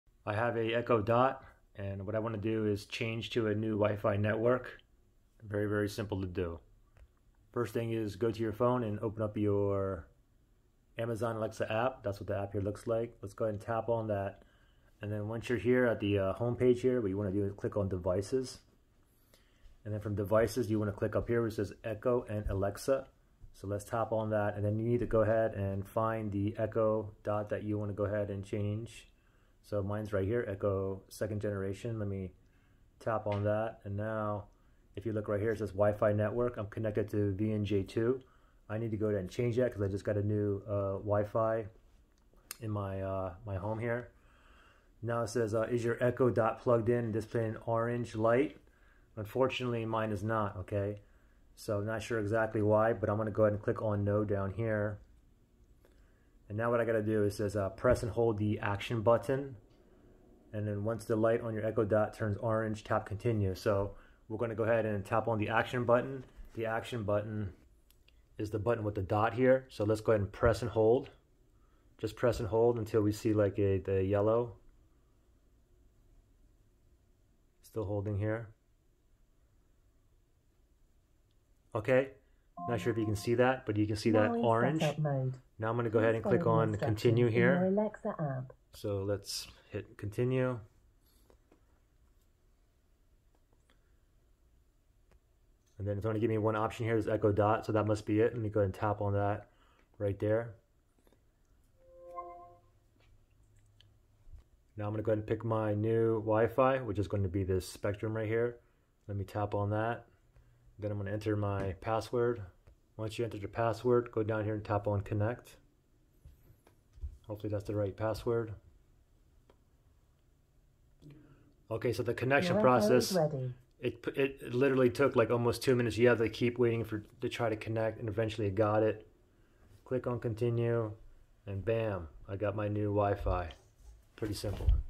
How to Change Wifi Network on Amazon Echo Dot (3rd gen, 2nd gen, 1st gen)
I show you how to change the wifi internet network on the Amazon Echo Dot. This will work on the Amazon Echo Dot 3rd generation, 2nd gen, and 1st gen. Hope this helps.